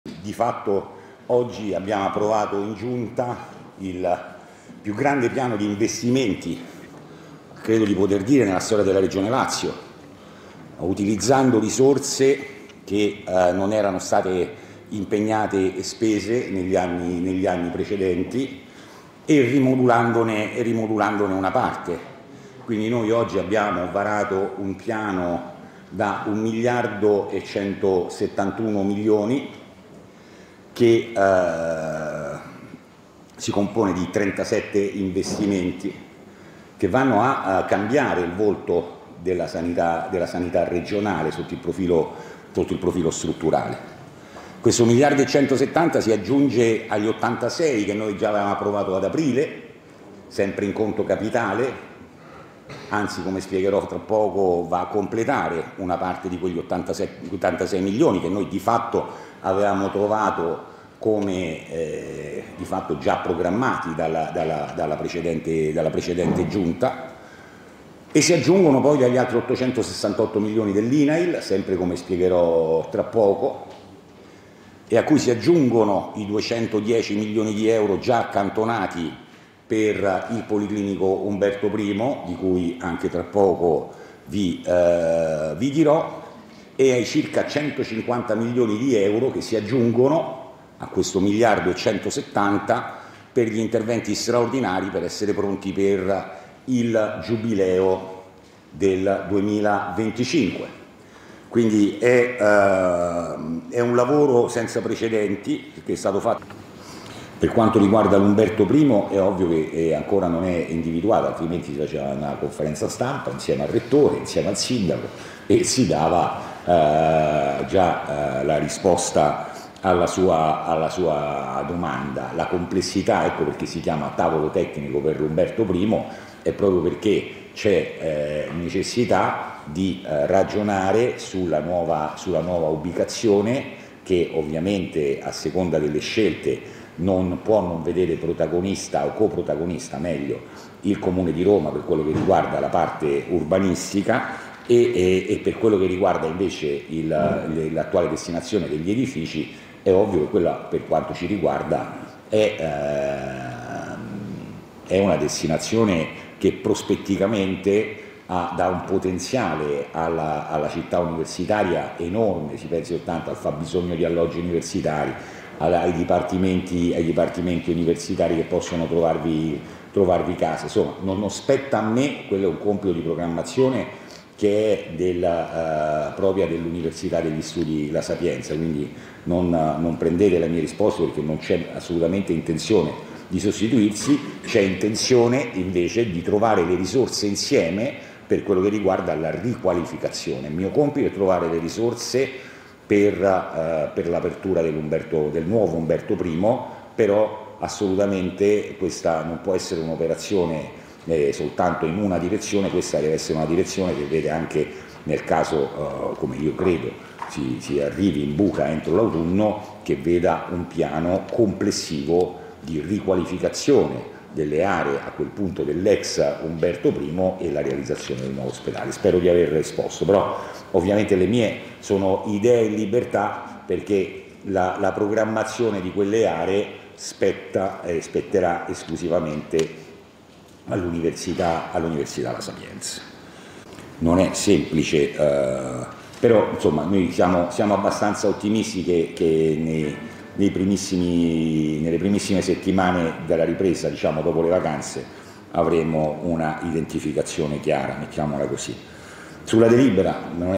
Rocca: "Al via più grande piano investimenti sanità da 1,2miliardi"
"In autunno nuova ubicazione policlinico Umberto I"

POLITICA (Roma). "Oggi abbiamo approvato in Giunta il più grande piano investimenti della storia della Regione Lazio utilizzando risorse non impegnate e non spese negli anni precedenti e rimodulandone una parte. Un piano da 1 miliardo e 171 milioni di euro che si compone di 37 investimenti che vanno a cambiare il volto della sanità regionale sotto il profilo strutturale". Lo ha detto Francesco Rocca, presidente della Regione Lazio, durante una conferenza stampa in materia di sanità, nella Sala Tevere di via Cristoforo Colombo. Ha aggiunto riguardo all'ubicazione del nuovo Umberto I: "Per il policlinico Umberto I abbiamo gia' accantonato 210 milioni e abbiamo aperto un tavolo tecnico per realizzare il nuovo policlinico con Laziodisco, Comune, Policlinico e naturalmente universita' La Sapienza. In autunno daremo notizia della nuova ubicazione, non e' pensabile che, nel 2023, un ospedale sia distribuito su 55 edifici. Quelle stesse devono poter trovare casa in una struttura moderna e razionale" (Stefano Chianese/alanews)

di fatto oggi abbiamo approvato in Giunta il più grande piano di investimenti credo di poter dire nella storia della regione Lazio o utilizzando le borse che non erano state impegnate e spese negli anni negli anni precedenti e rimodulando ne eri modulandone una parte quindi noi oggi abbiamo imparato un piano da un miliardo e 171 milioni che si compone di 37 investimenti che vanno a cambiare il volto della sanità della sanità regionale sotto il profilo sotto il profilo strutturale questo miliardo 170 si aggiunge agli 86 provato ad aprire sempre in conto capitale  Anzi come spiegherò tra poco va a completare una parte di quegli 8786 milioni che noi di fatto avevamo trovato come hai fatto già programmati dal dalla precedente dalla precedente Giunta  e si aggiungono poi agli altri 868 milioni dell'INAIL sempre come spiegherò tra poco e a cui si aggiungono i 210 milioni di euro già accantonati per il Policlinico Umberto Primo di cui anche tra poco vi vi dirò e hai circa 150 milioni di euro che si aggiungono a questo miliardi 170 per gli interventi straordinari per essere pronti per il Giubileo del 2025 quindi è è un lavoro senza precedenti che è stato fatto per quanto riguarda Umberto Primo è ovvio che è ancora non è individuata altrimenti faceva la conferenza stampa insieme al rettore cioè non si fidava già la risposta alla sua alla sua domanda la complessità è quello che si chiama tavolo tecnico per Umberto Primo è proprio perché  necessità di ragionare sulla nuova sulla nuova ubicazione che ovviamente a seconda delle scelte non può non vedere protagonista Alcott sta meglio il Comune di Roma per quello che riguarda la parte urbanistica È per quello che riguarda invece il attuale destinazione degli edifici è ovvio quello per quanto ci riguarda è una destinazione che prospetticamente ad alto potenziale alla città universitaria enorme ci pensi 80 al giorno di alloggi universitari ai dipartimenti ai dipartimenti universitari che possono trovarvi trovarvi caso sono spetta a me quello è un compito di programmazione che è della propria dell'Università degli Studi La Sapienza quindi non Non prendete le mie risposte perché non c'è assolutamente intenzione di sostituirsi c'è intenzione invece di trovare le risorse  per quello che riguarda la riqualificazione mio compito trovare le risorse per per l'apertura del Umberto del nuovo Umberto Primo però assolutamente questa non può essere un'operazione soltanto in una direzione questa veste una direzione che vede anche nel caso io credo Si si arrivi in buca entro l'autunno che veda un piano di riqualificazione delle aree a quel punto del Lezza Umberto Primo e la realizzazione del nuovo ospedale Spero di aver risposto Però ovviamente le mie sono idee Libertà perché la programmazione di quelle aree spetta e spetterà esclusivamente all'università all'università La Sapienza non è semplice però domani diciamo siamo abbastanza ottimisti che che nei nei primissimi nelle primissime settimane dalla ripresa Diciamo dopo le vacanze Avremmo una identificazione  Chiama rossi sulla delibera